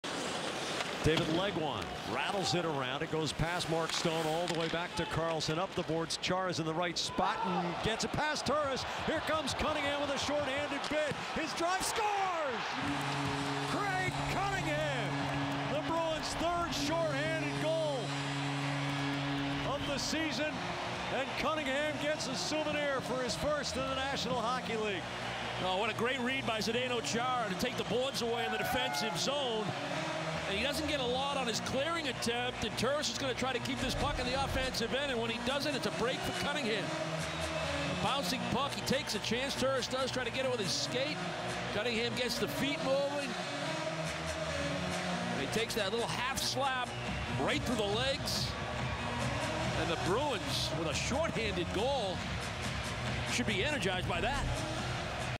Craig Cunningham's 1st NHL goal 12/13/14
Craig Cunningham scores his first NHL goal, a shorthanded goal vs the Ottawa Senators, on December 13, 2014.